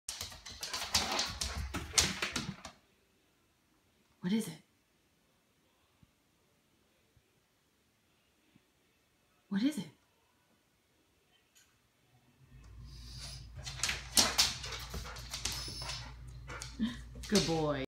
WINDOW SILL NIGHTMARE
WINDOW SILL NIGHTMARE! ARMANI CGC BN RN loves to check out the squirrels all day and has ruined his owner Tina Cummins window sill but she is happy because he is so darn CUTE!!! He's 20 mos old owned and handled by Tina Cummins in AR. By health tested AKC CH's GCH War Bouviator Ekabu's Mercury V Dewerckers RAE,HSAds,CAA,VBCH"Sterling" X Dior. Debbie Bevel Potter and Kim Morissette.